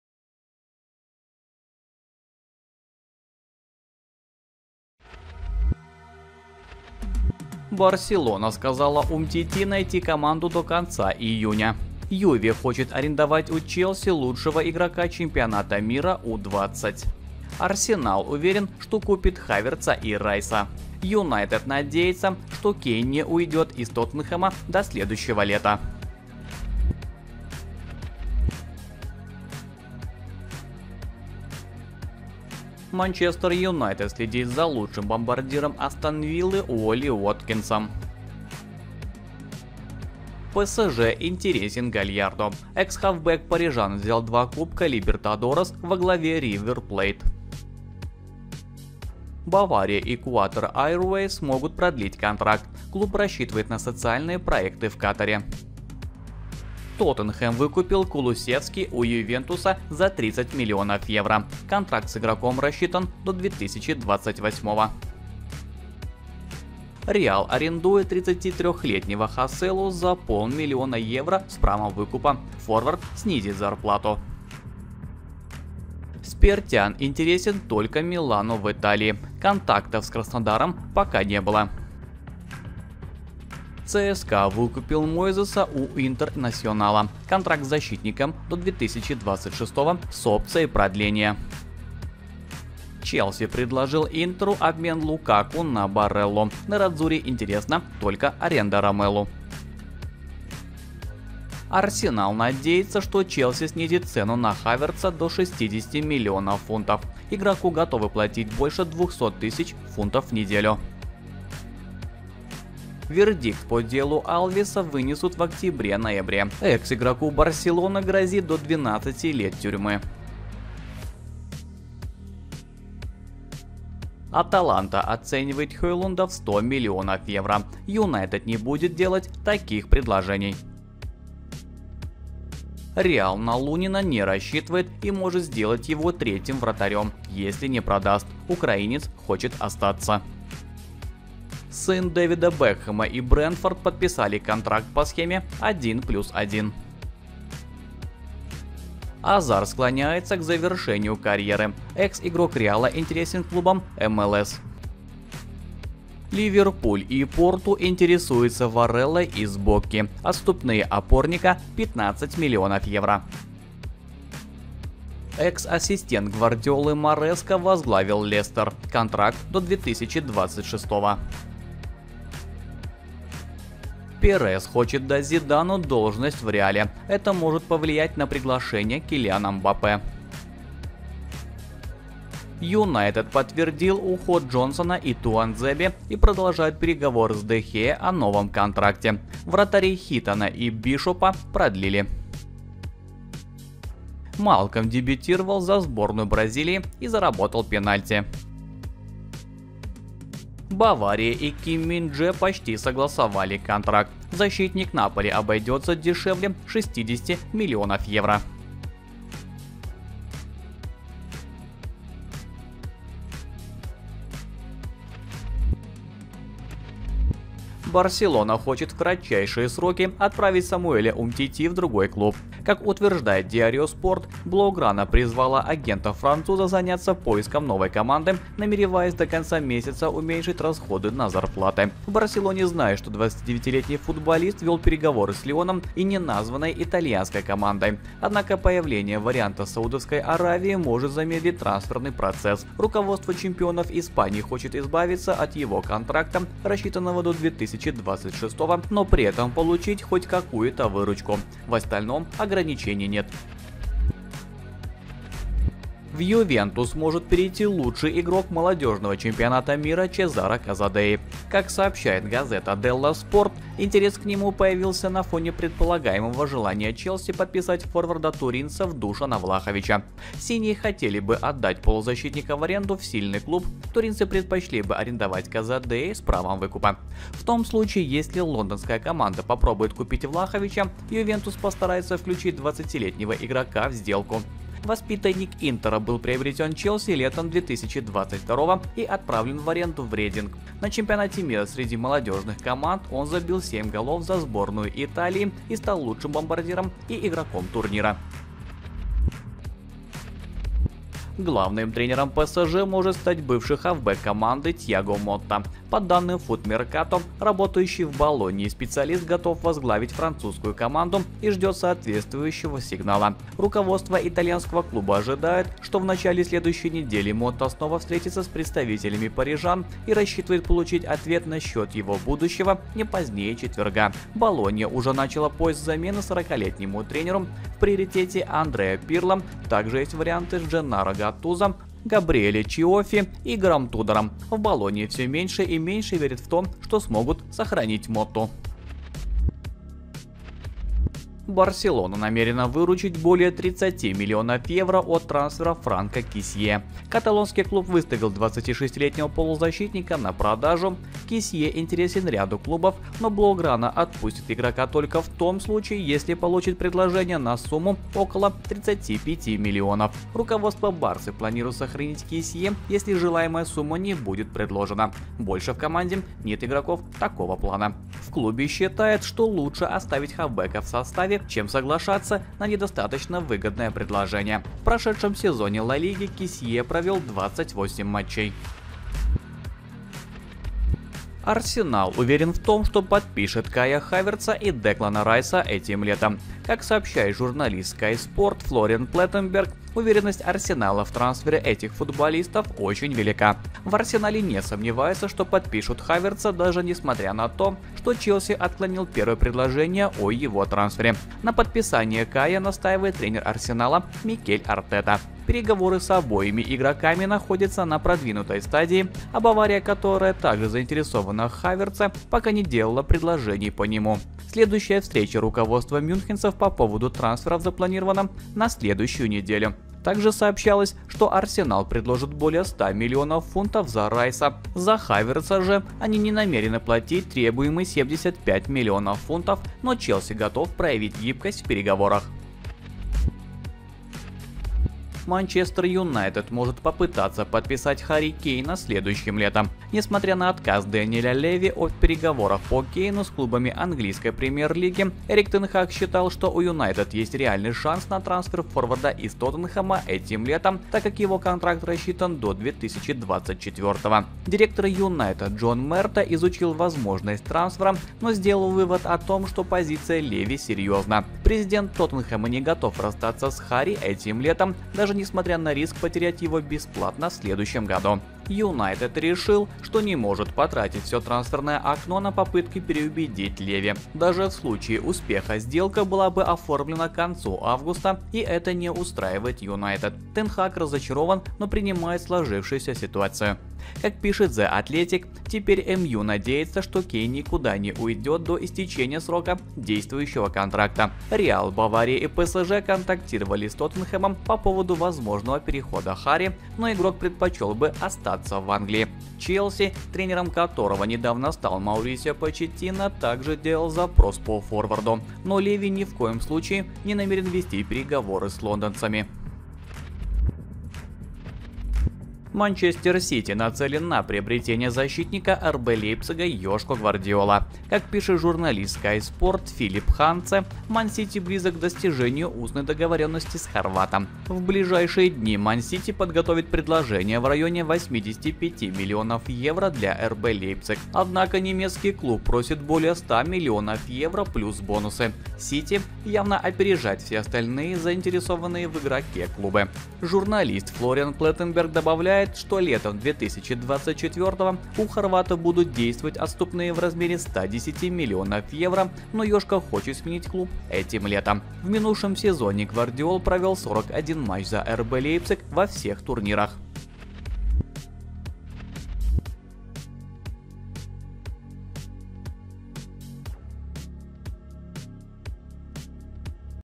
МЮ ЕЩЕ ХОЧЕТ КЕЙНА | ГОНКА ЗА ГВАРДИОЛОМ | ТРАНСФЕРНЫЕ СЛУХИ 2023
00:00 👉 Превью
00:27 📢 SHORTS NEWS:
👉 «Реал» на Лунина не рассчитывает
👉 «Аталанта» оценивает Хейлунда в 100 млн евро
👉 Азар склоняется к завершению карьеры
👉 «Ливерпуль» и «Порту» интересуются Варелой
👉 Экс-ассистент Гвардиолы
👉 Перес хочет дать Зидану должность
👉 «Тоттенхэм» выкупил Кулусевски

📢 НОВОСТИ 📃:
3:35 👉 «Барселона» сказала Умтити найти команду
4:14 👉 «Юве» хочет арендовать лучшего игрока ЧМ U20
5:00 👉 ​Мотта готов возглавить «ПСЖ»
5:39 👉 «Барселона» не продаст Кессиэ
6:20 👉 ​«Арсенал» уверен, что купит Хавертца и Райса
7:13 👉 «МЮ» надеется, что Кейн не уйдет
8:30 👉 «Ман Сити» даст 80-85 млн евро за Гвардиола